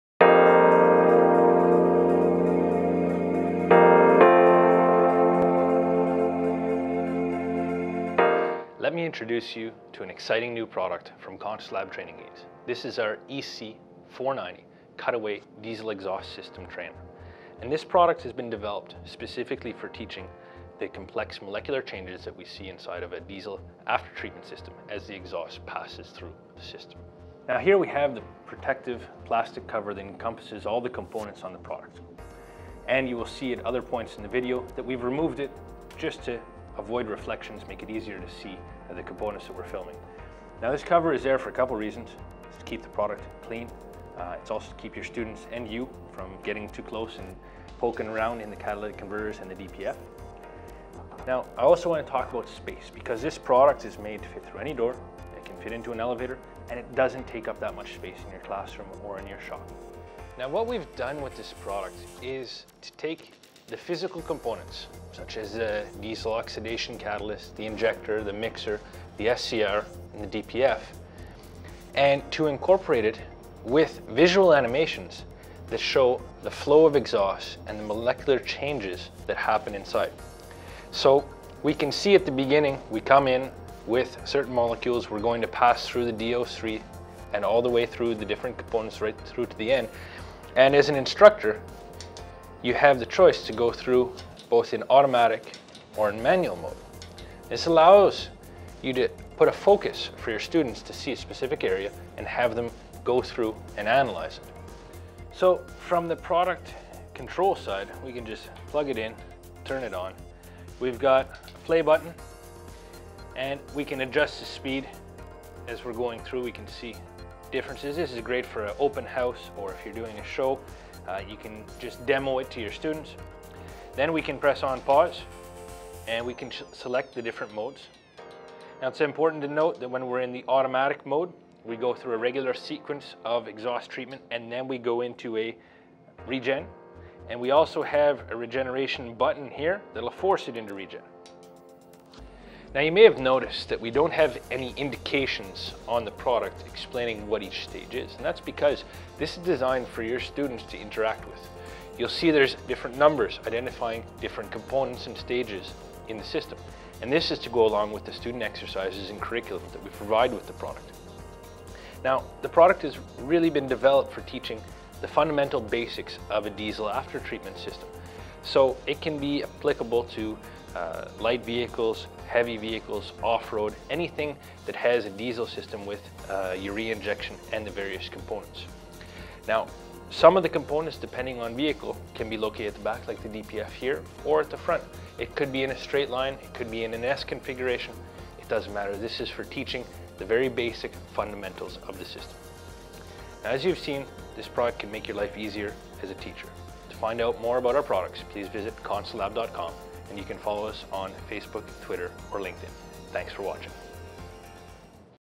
EC-490 CUTAWAY DIESEL EXHAUST AFTERTREATMENT SYSTEM
The ConsuLab EC-490 is a cutaway diesel exhaust system trainer that allows students to view the components of a diesel exhaust aftertreatment system using LED’s and electronic controls to help learners understand the function, and effect on, diesel engine exhaust gases following the combustion cycle. The trainer uses multiple colored LED’s to represent the gases that flow in the exhaust system and, demonstrates the chemical reactions within each of the aftertreatment processes.